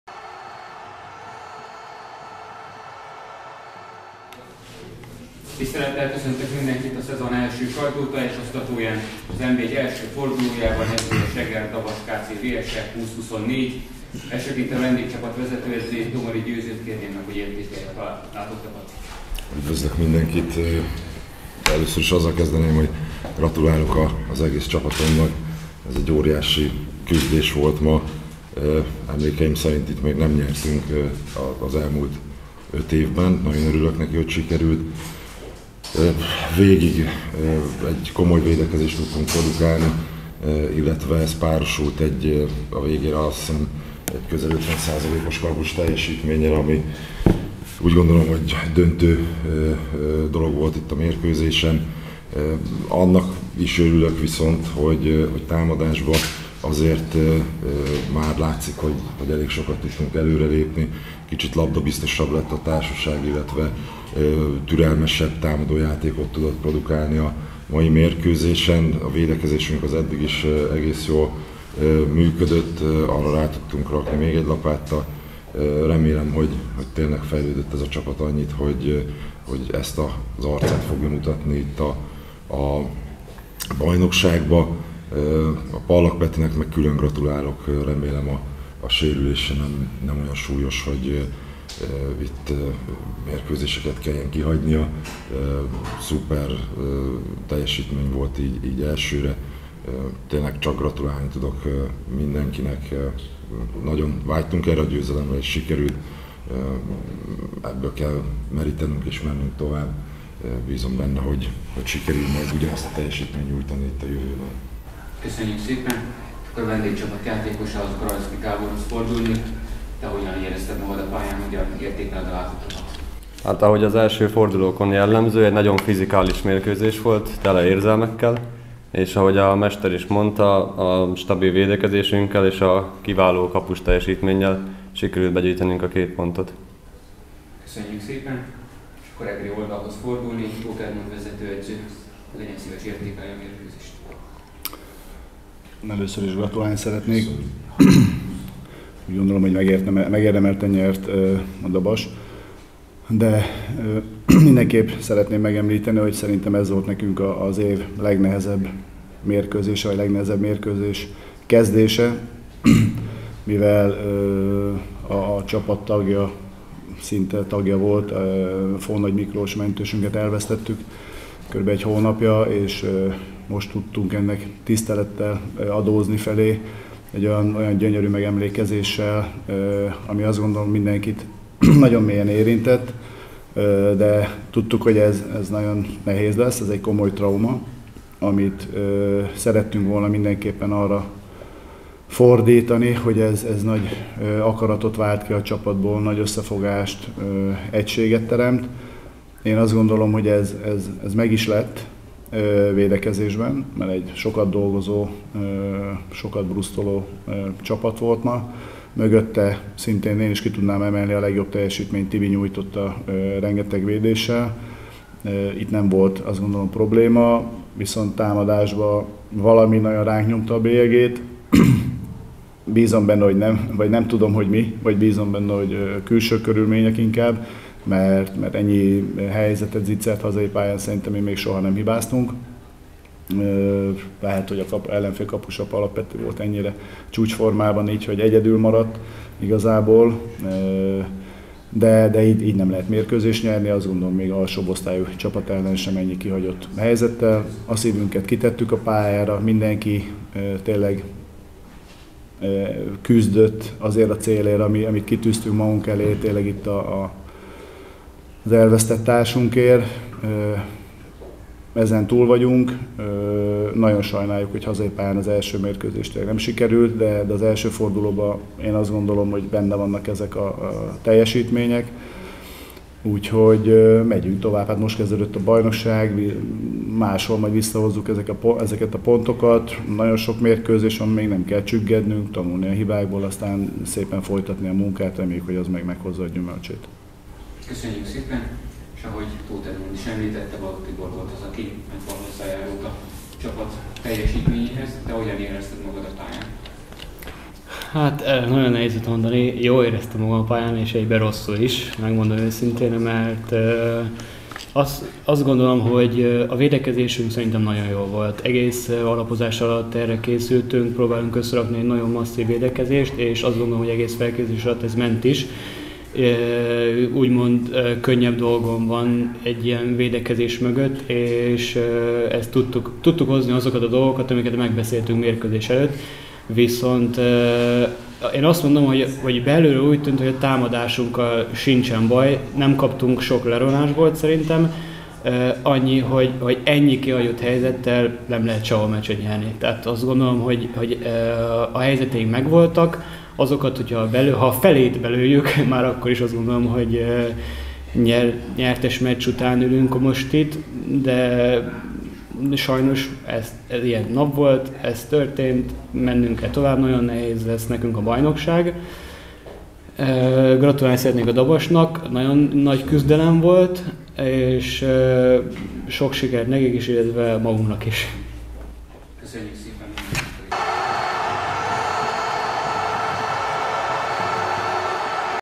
Az SBS-Eger–Dabas KC VSE elleni bajnoki mérkőzést (20–24) követő sajtótájékoztató
SBS - Eger